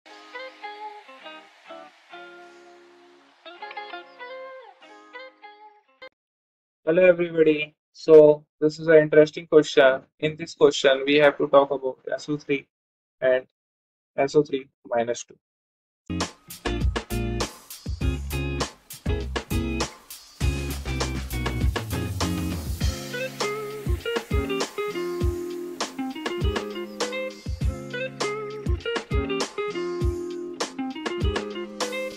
In which one of the following the best Lewis structure a resonance structure? SO3 SCO (C = central …
In which one of the following the best Lewis structure a resonance structure? SO3 SCO (C = central atom) SO32

UnderstandingChemicalBonding:TheKeytoMolecularStructure